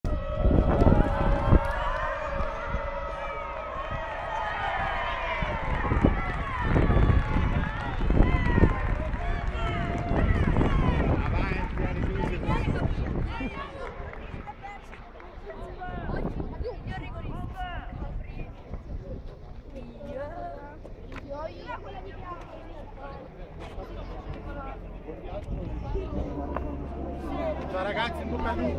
Under 12, Parma vincitore del Memorial Beghi: esultanza in tuffo
Stadio Garilli, Piacenza, Sabato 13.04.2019